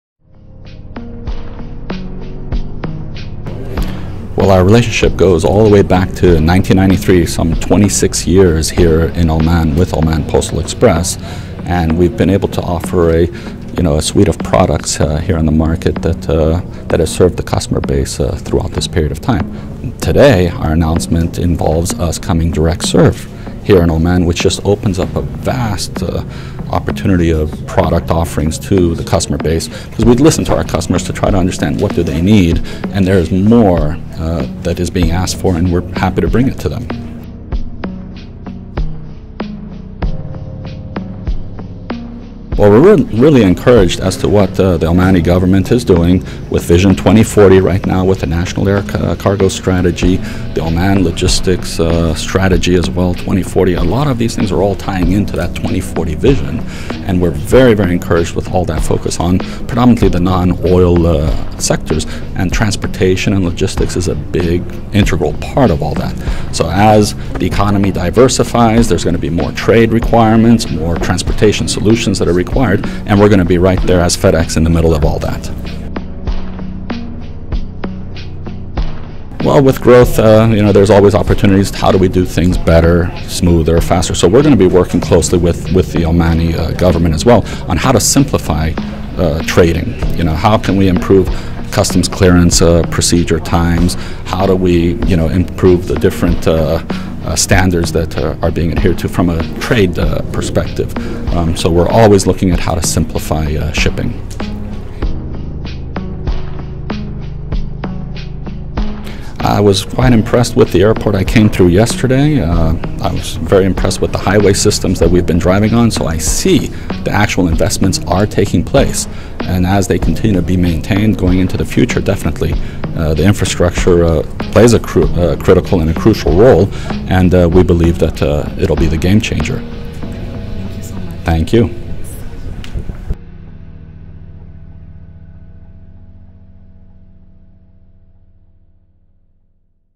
Taarek Hinedi of FedEx on their recent expansion in Oman
Business Live ME interviews Taarek Hinedi - FedEx Express Vice President Middle East and North Africa Operations.